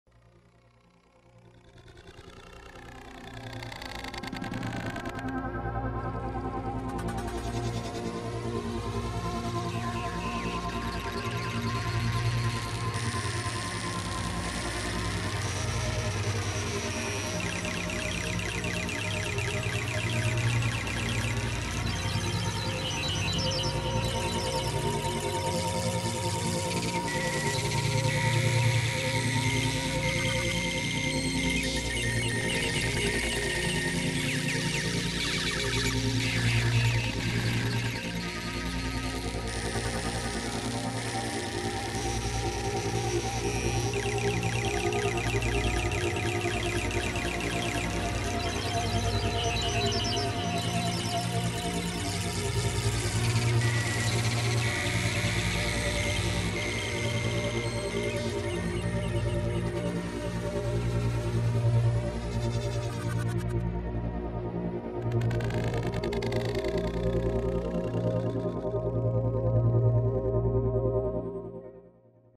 [3D SBS VR] Memory from DiMiTris room (audio/visual experiment v4) [SEIZURE WARNING]
Trying to figure out this 3D VR space once again.
Headphones and VR headset Highly recommended for this one.

Trying to piece together brief memories from past DMT experiences.
Remember, this is not "done-end-piece", it's just an iteration to gather feedback so the next piece can be more immersive.
As always, I'd like to hear your feedback.